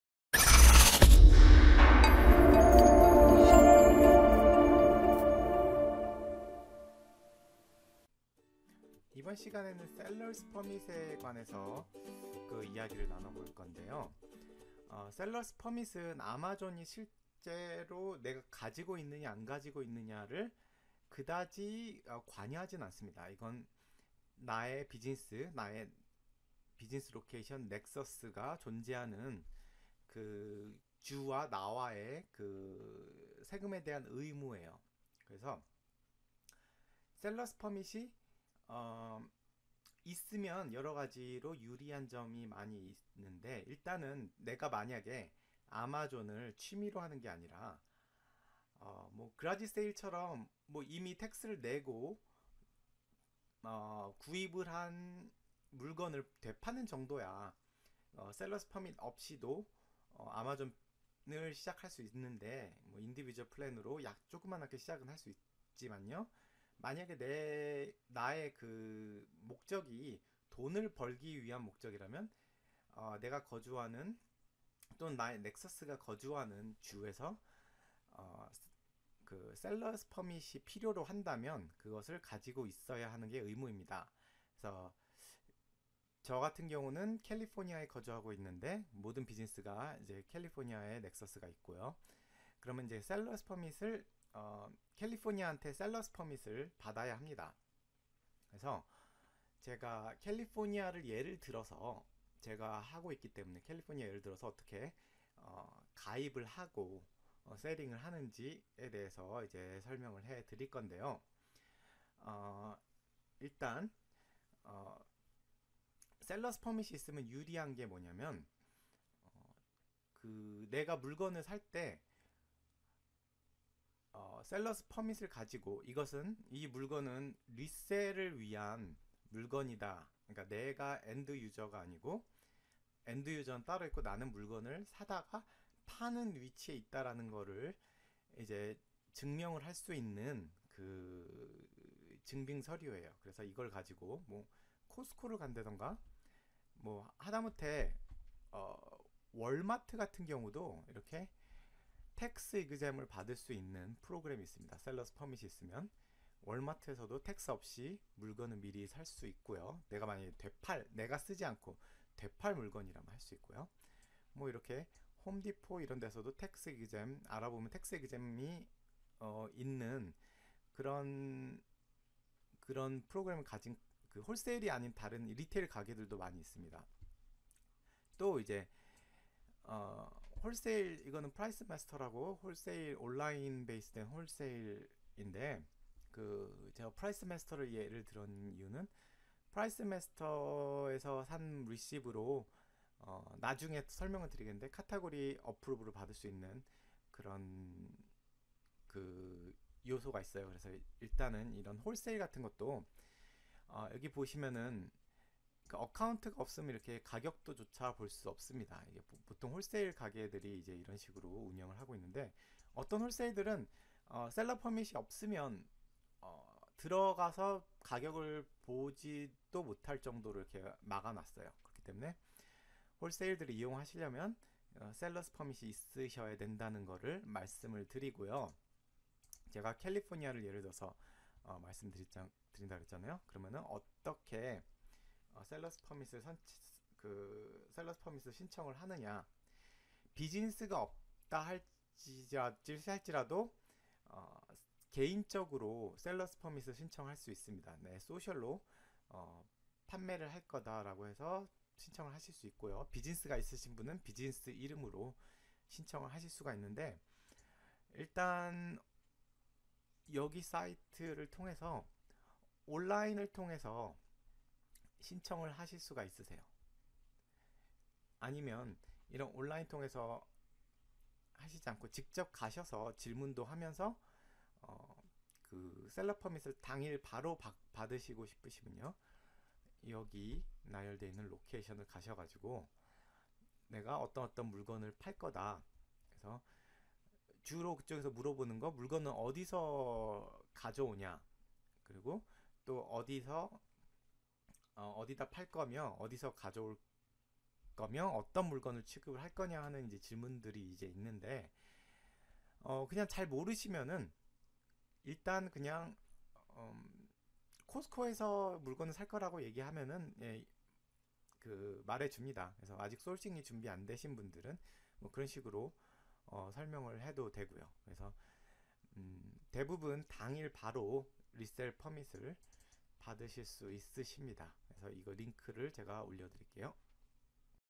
아마존 FBA 비즈니스 - Seller's Permit 에 관해서 (Sales Tax 정리 2탄)
California Seller Permit 온라인 등록

3. 아마존 FBA 비즈니스 - State Tax Setting 아주 중요 합니다. (Sales Tax 정리 3탄)